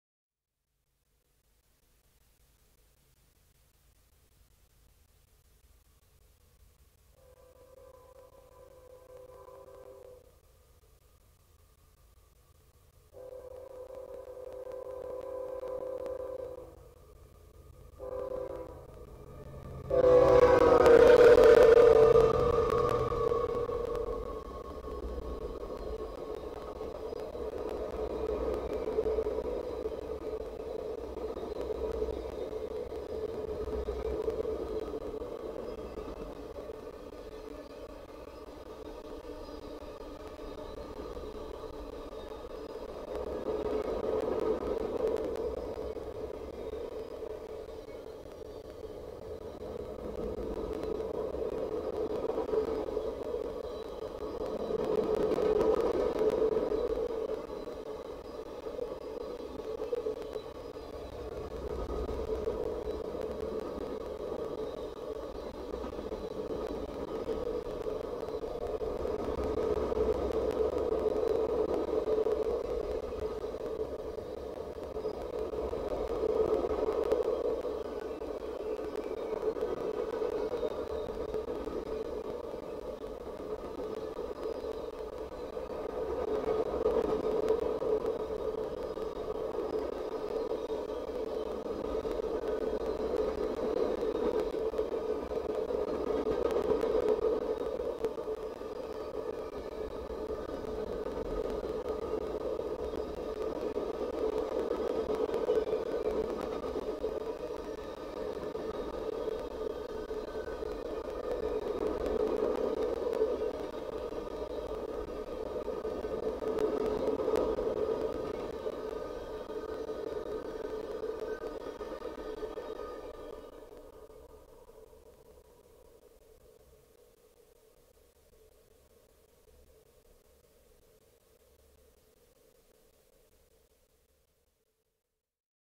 BNSF GEVO leads eastbound Manifest with Awesome lashup! - La Plata, MO
5/25/17 - Eastbound Manifest with an awesome lashup, including 2 CSX GEVO's, H3 SD40-2R, and former UP GP50!